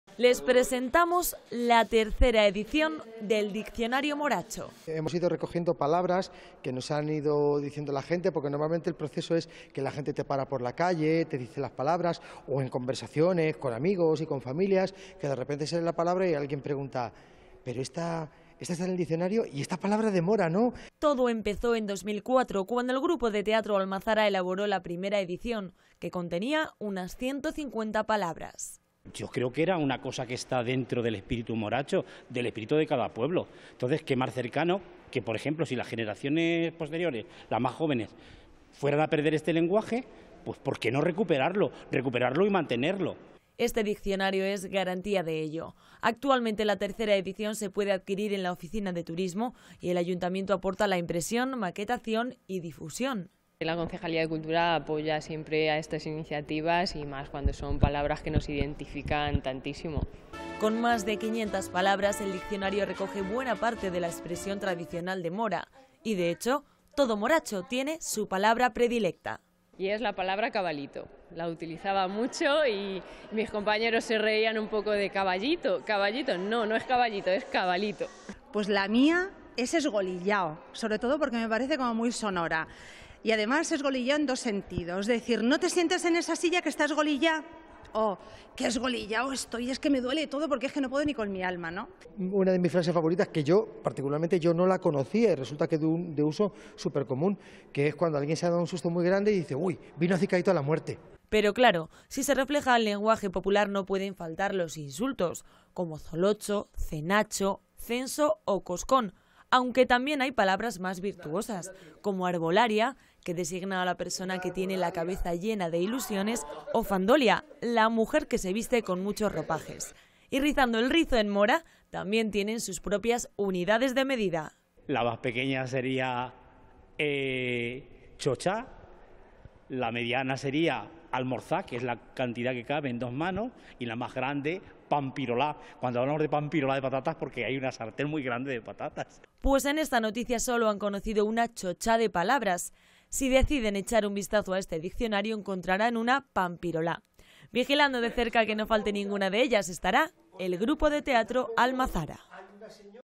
El popular "Diccionario Moracho" ya va por su Tercera Edición
Descubrimos las nuevas palabras y expresiones de este peculiar diccionario de la mano de sus creadores, el Grupo de Teatro Almazara. La Regional /Teletoledo